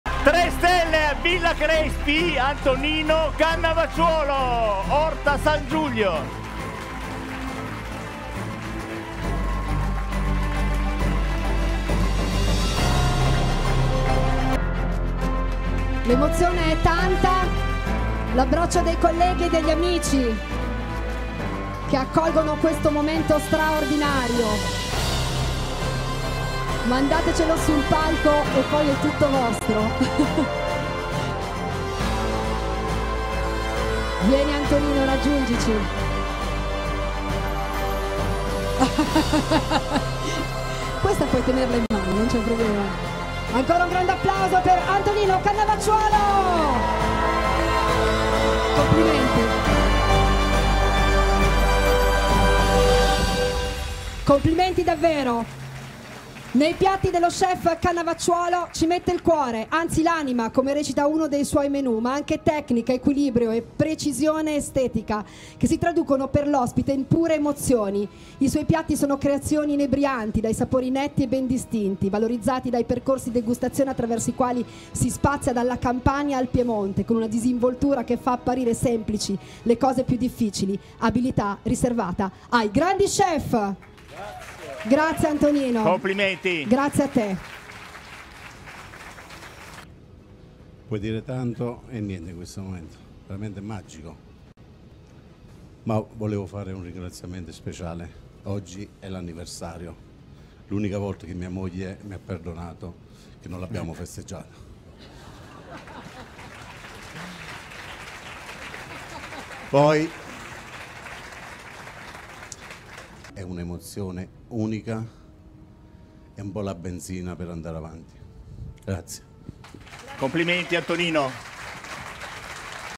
Guida Michelin, tre stelle a Cannavacciuolo: «Momento magico, lo dedico a mia moglie»
Antonino Cannavacciuolo entra nell'Olimpo dei ristoranti tre stelle Michelin definendo così la magnifica dozzina della cucina italiana: sono stati tutti confermati gli undici ristoranti tre stelle anche nella guida Michelin Italia 2023 . Il commento dello chef: «E' un momento magico, - ha commentato a caldo Antonino - oggi è anche l'anniversario del matrimonio e mia moglie mi ha perdonato per questo festeggiamento a distanza. Il nostro è un progetto partito 20 anni fa, ci saranno altre aperture - ha preannunciato l'imprenditore - e questa enorme nuova emozione è la benzina per andare avanti»